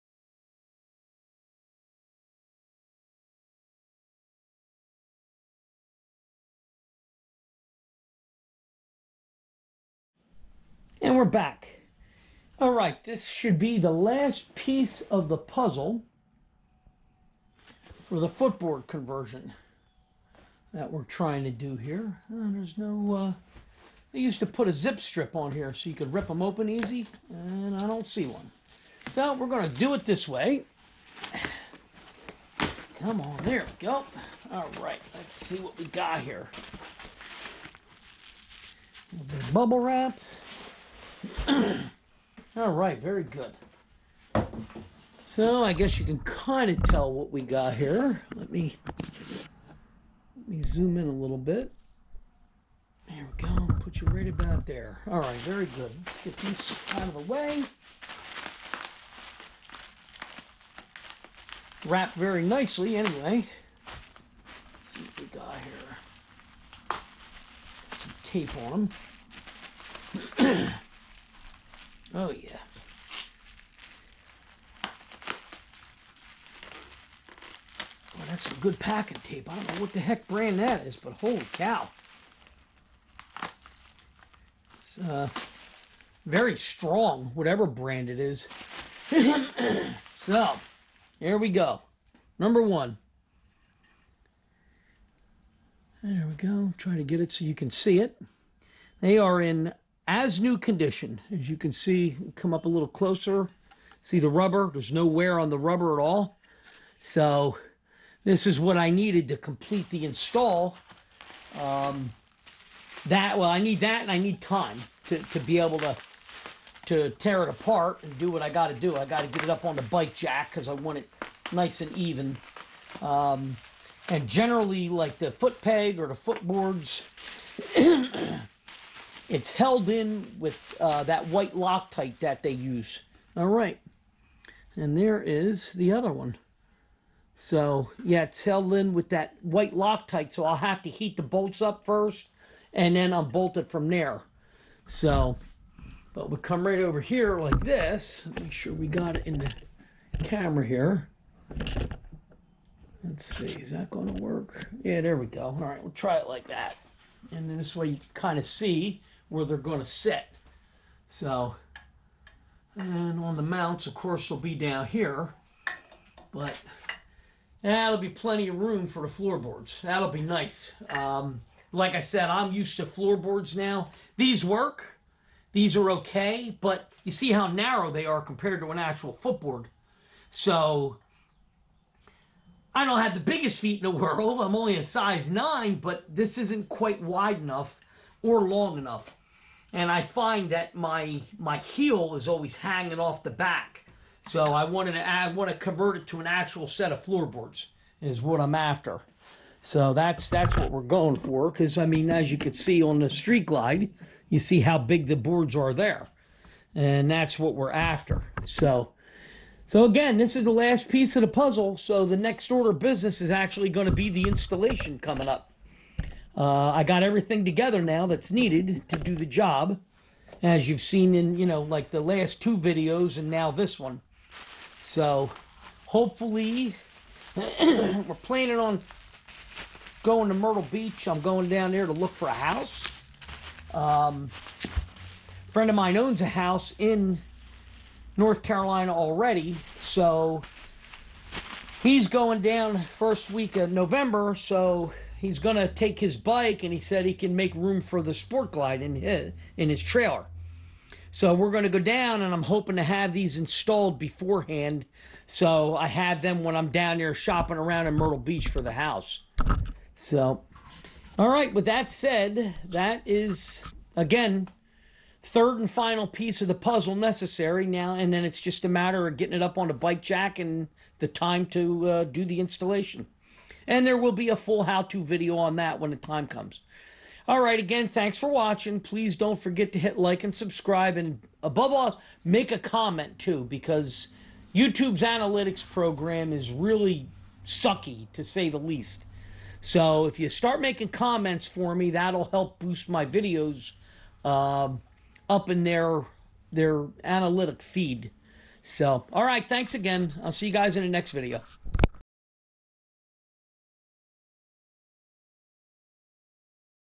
2018 Sport Glide FLSB 3rd and FINAL piece needed for conversion
this is the unpacking of the 3rd and final items needed for the floorboard conversion on my  2018 Harley Sport Glide ... they are OEM contemporary style floorboards in as new condition that i picked up on ebay for $140 shipped which was a great deal...

so now all i need is some time to do the conversion itself and make the HOW TO video

please dont forget to hit like and subscribe if you want to see the conversion video when i get it done and uploaded to the channel  PLEASE comment if this video helps you out as it will help me with you tubes analytics program